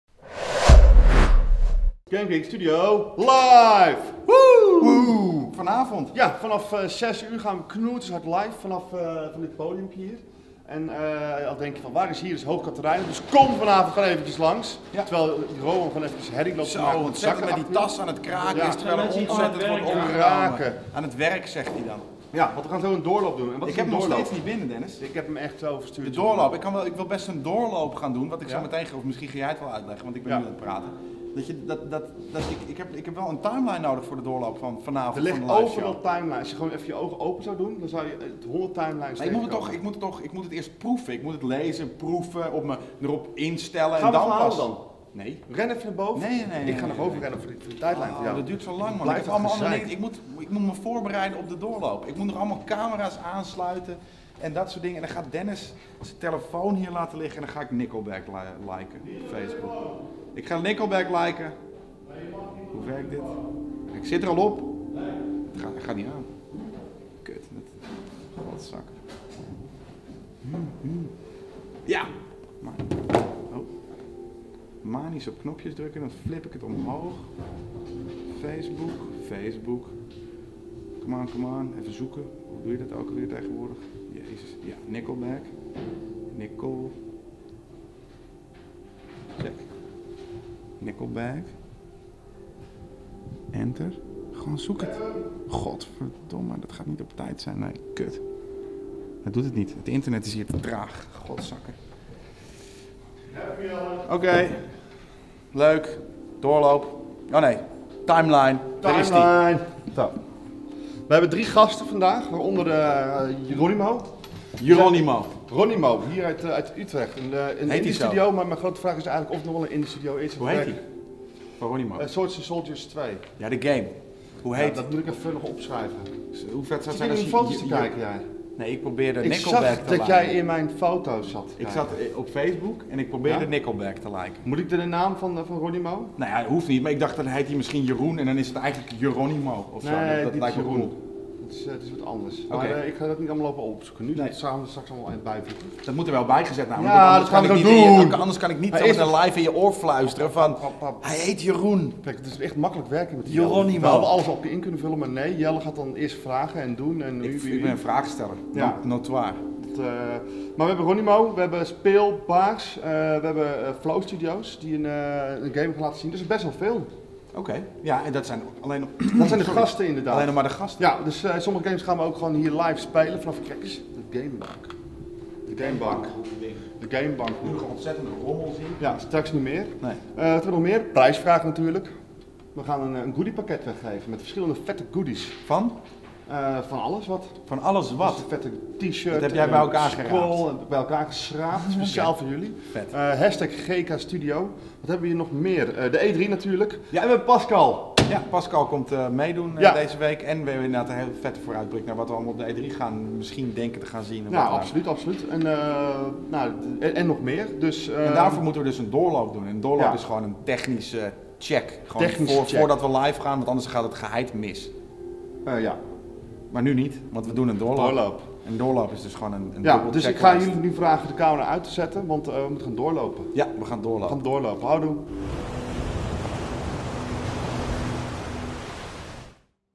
Jelle en Dennis doen de doorloop van Gamekings Studio Live
Jelle en Dennis geven een kijkje achter de schermen in Gamekings Studio. Daarnaast probeert Jelle Dennis zijn Facebook te saboteren. Lukt het hem?